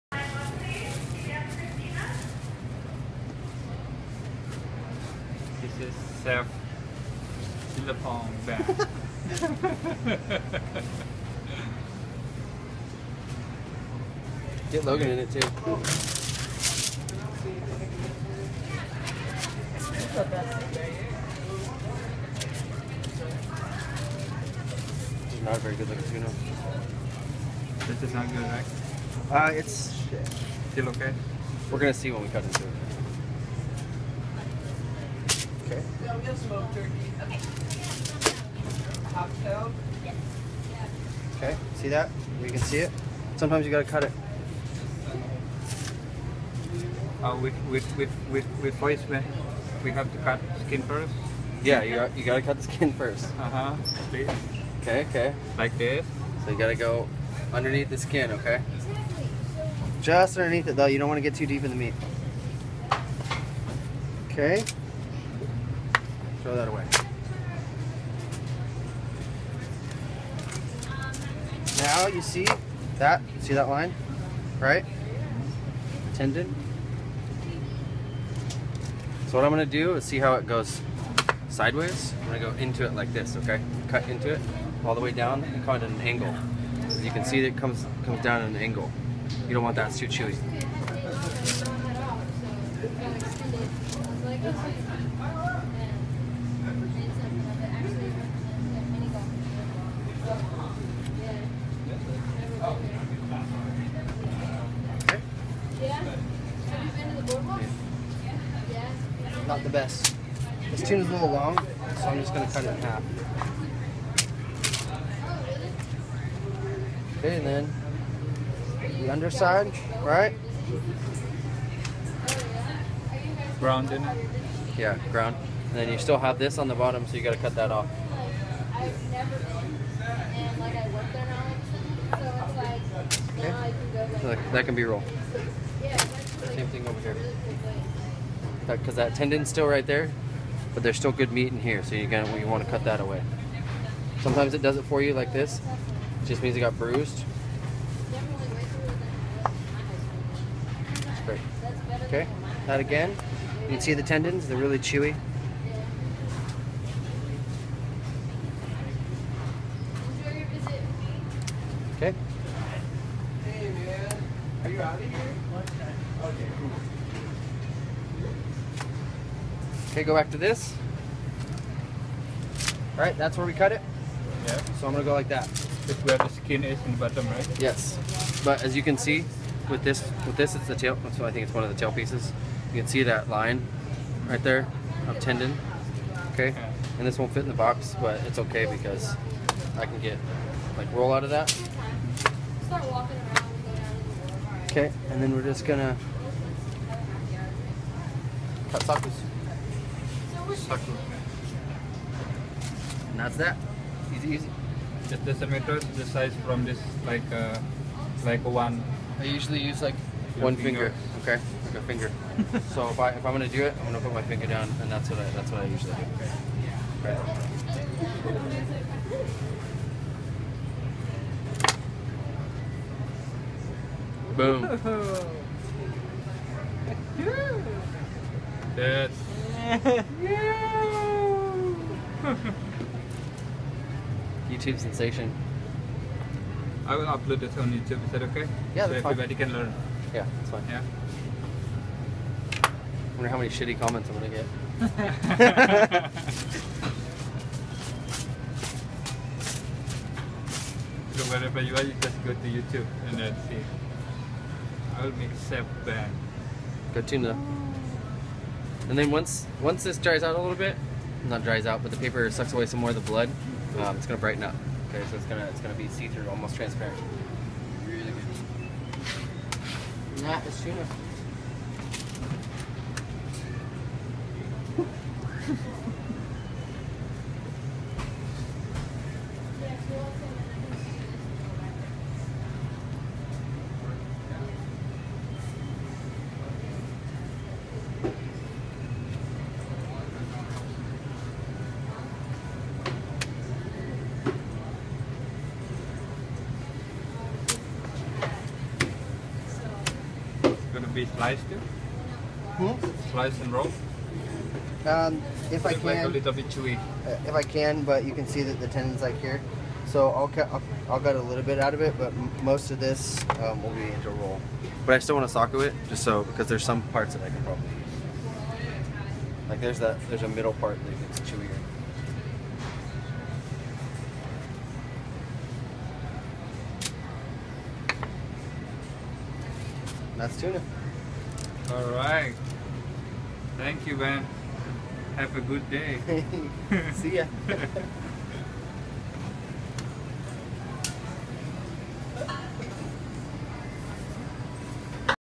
How to cut Tuna
Chef Ben show how to cut Tuna, thanks !